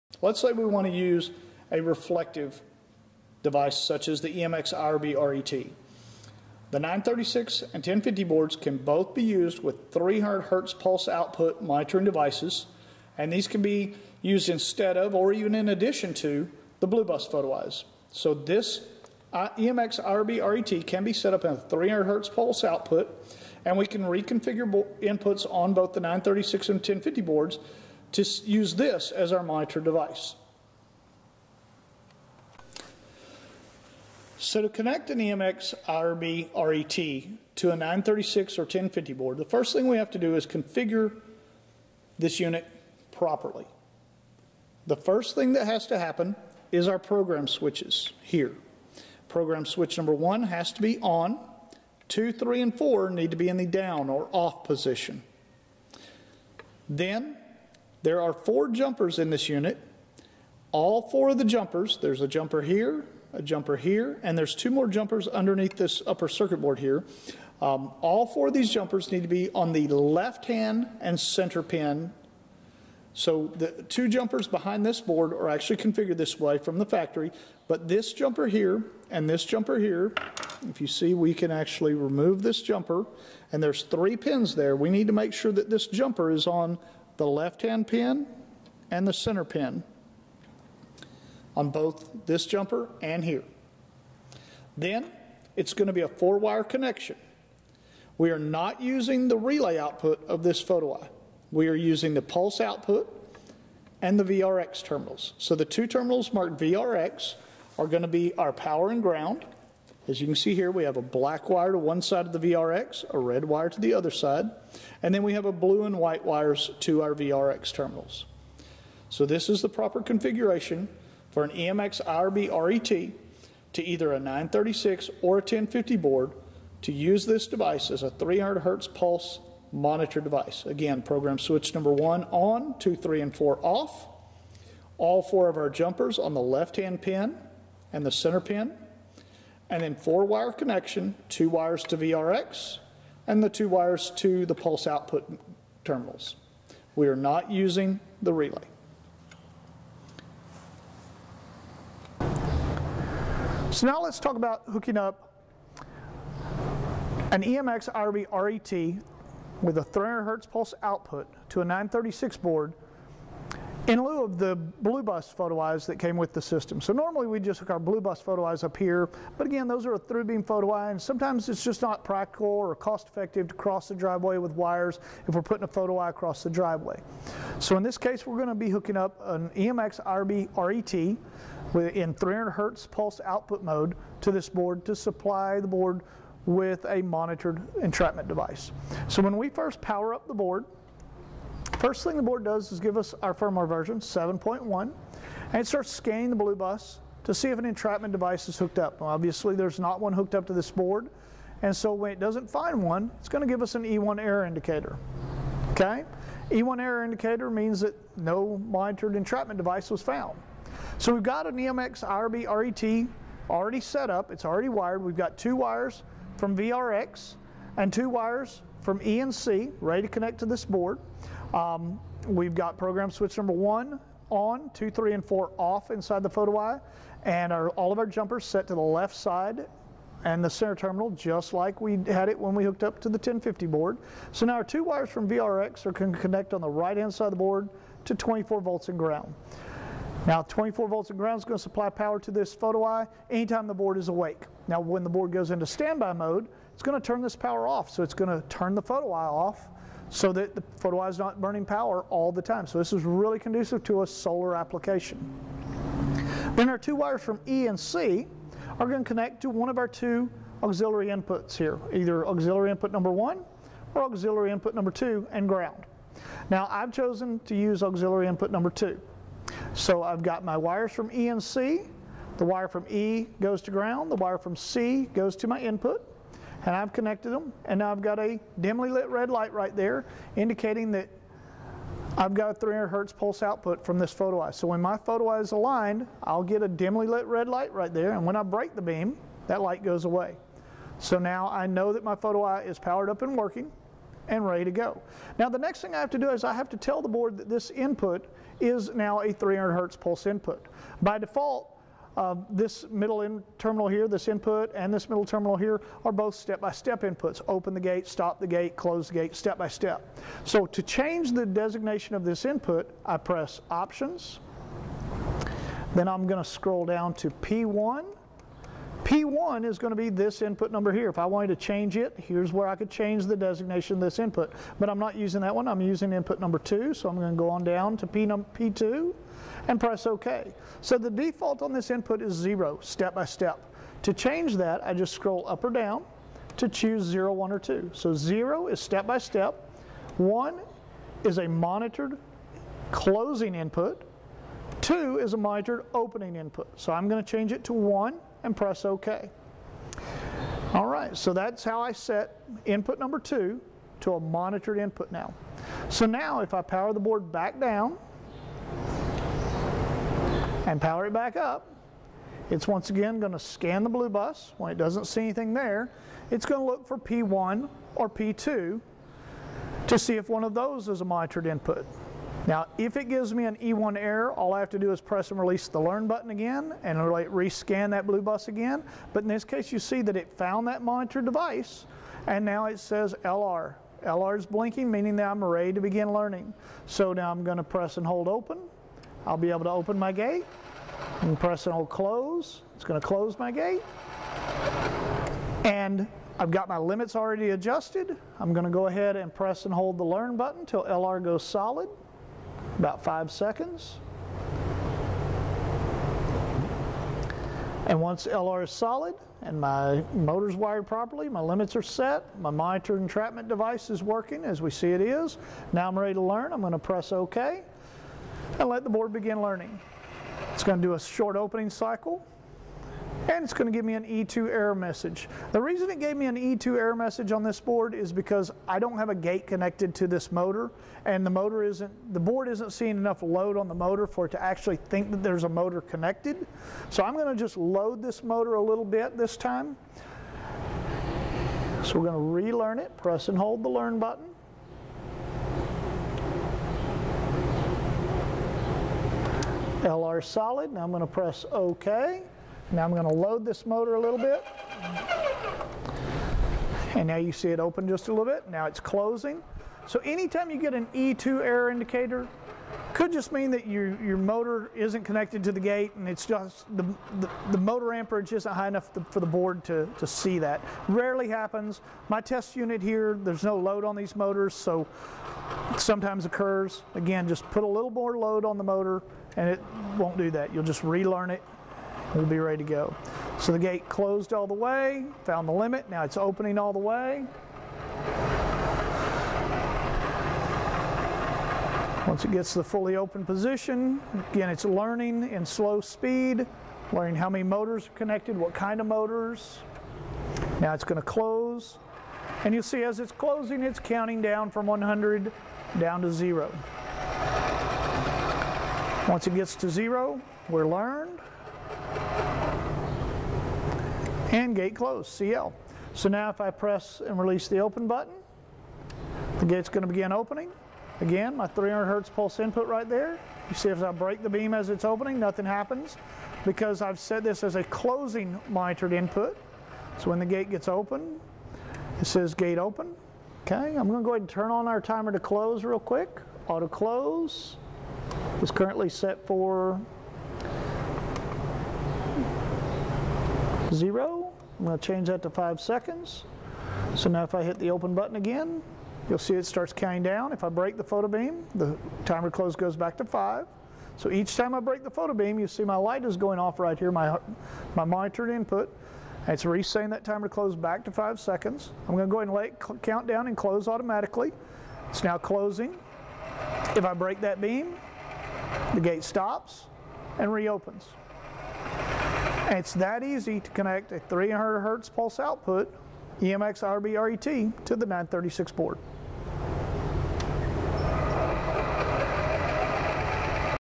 Connecting EMX IRB-RET to Nice 936US Control Board
Learn how to connect the EMX IRB-RET retroreflective photo eyes to a Nice 936US control board.